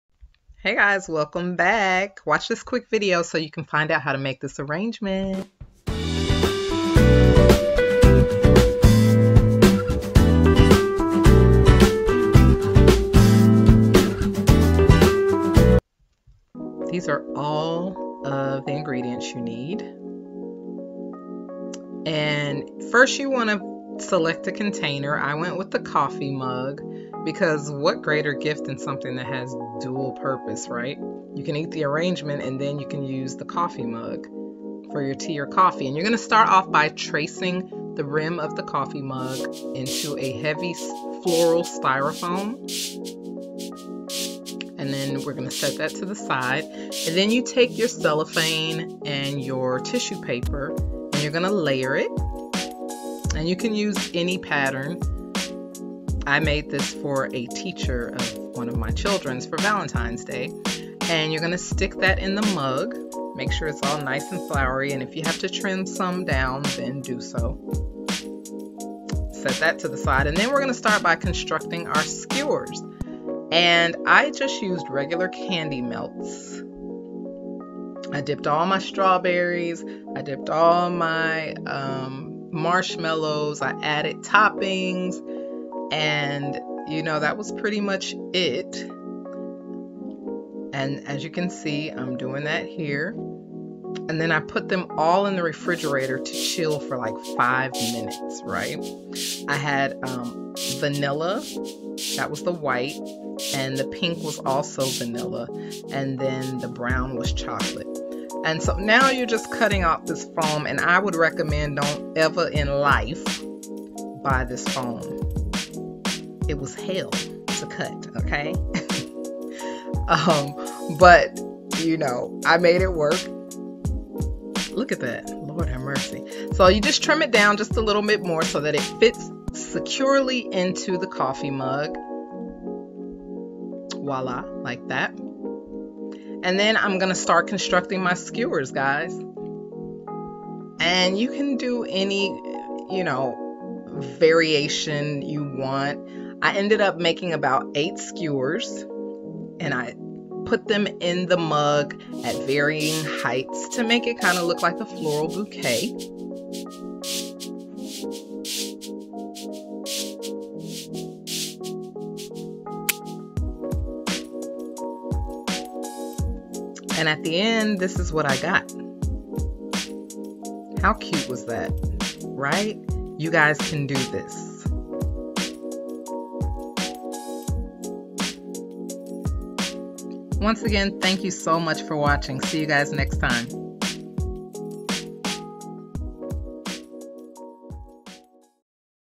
DIY Edible Arrangement| How To Make An Edible Fruit Arrangement Bouquet
Hello to the Divine 49!!  Learn how to make this quick easy edible arrangement!  It's a fun easy project I hope you will try!  Thanks for your continued support.

XOXO
Felicia

You will need:

Stawberries
Marshmellows
Candy Melts
Celephane
Tissue Paper
Bamboo Skewers
Floral Foam
Coffee Mug